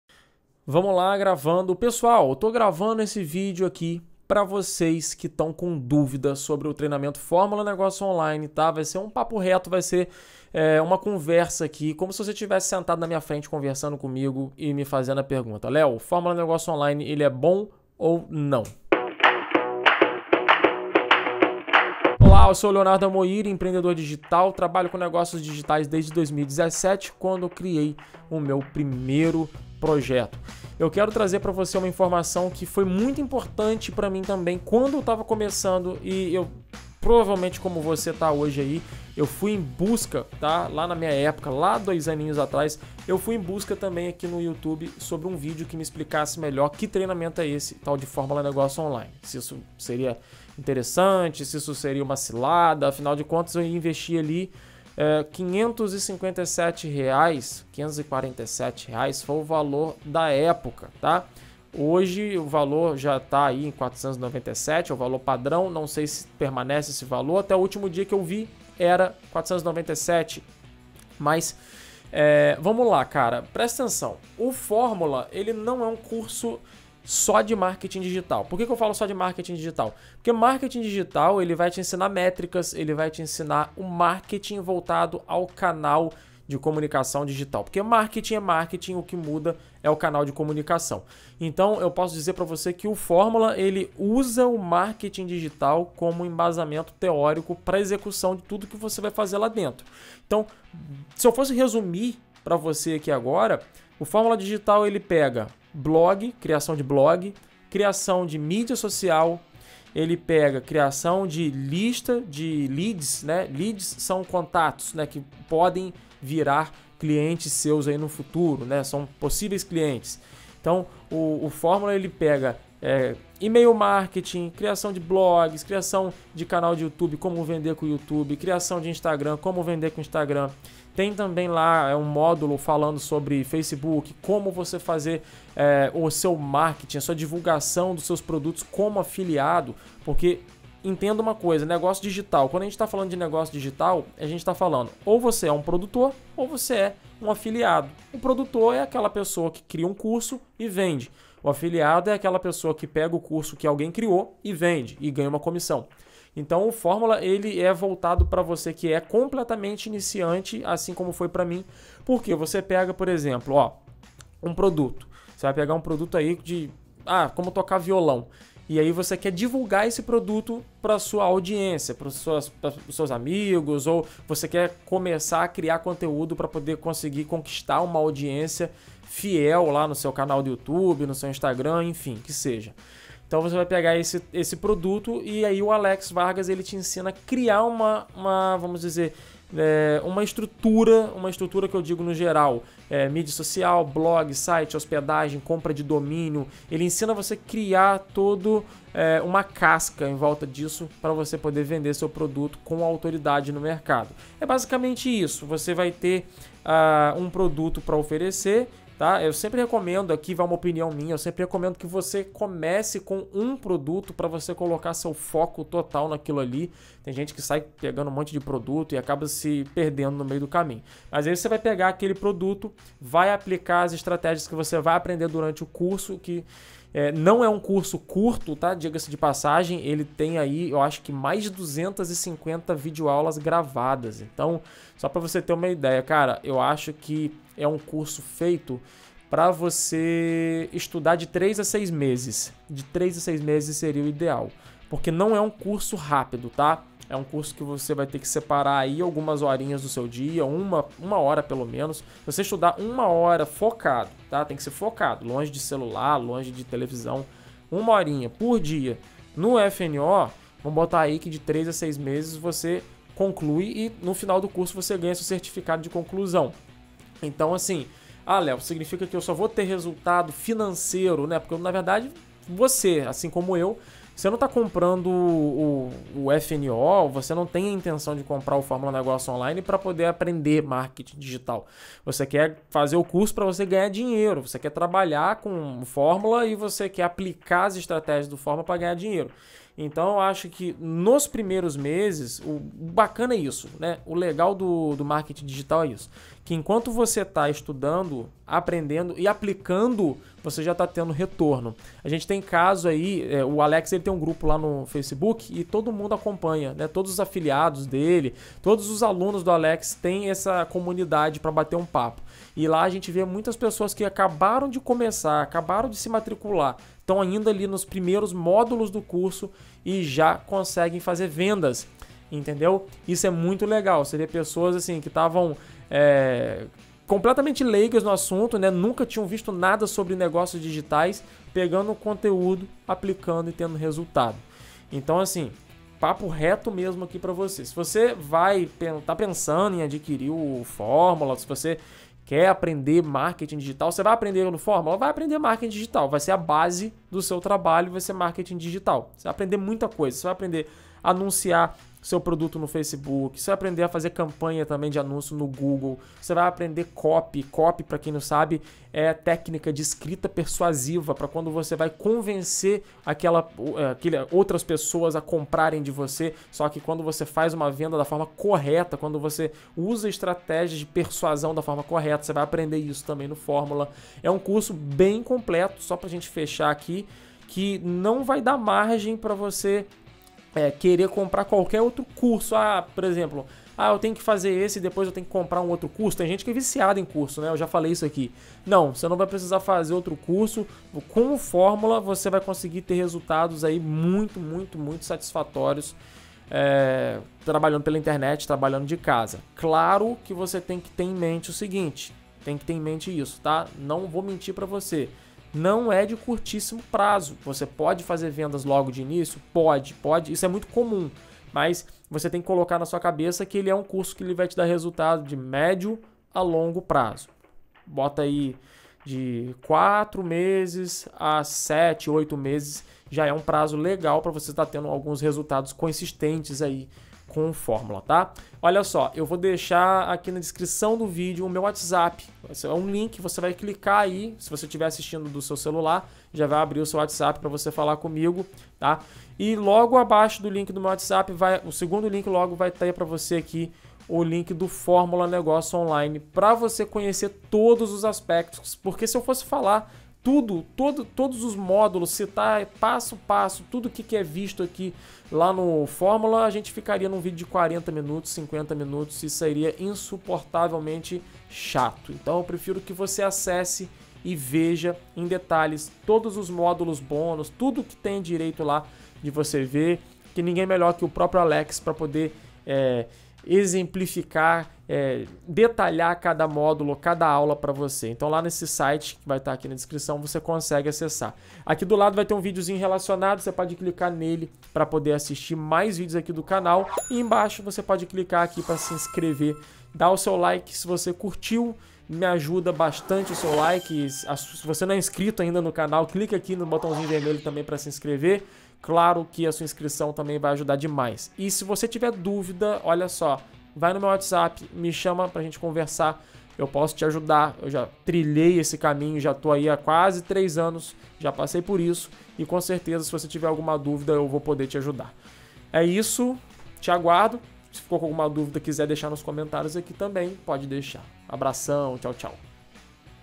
O FÓRMULA NEGÓCIO ONLINE FUNCIONA? TUDO O QUE VOCÊ PRECISA SABER ANTES DE SER ALUNO DO ALEX!
Muitas pessoas me perguntam se o curso Fórmula Negócio Online realmente funciona. Eu sempre digo que não há uma resposta certa. Sim e não!

O Fórmula Negócio Online funciona para quem se empenhar, se dedicar, estudar e - PRINCIPALMENTE - aplicar o conhecimento adquirido.

Se você é uma daquelas pessoas que está insatisfeita com sua condição de vida atual, seja financeiramente ou no pouco tempo livre que dispõe para viver uma vida de qualidade, o Fórmula Negócio Online certamente irá abrir seus horizontes.

Se ficar com dúvidas sobre o curso, deixe um comentário logo abaixo ou me chama no WhatsApp.

✔️ E-book GRATUITO: Aprenda as 5 técnicas que profissionais do marketing usam pra faturar MILHARES DE REAIS e domine o seu mercado.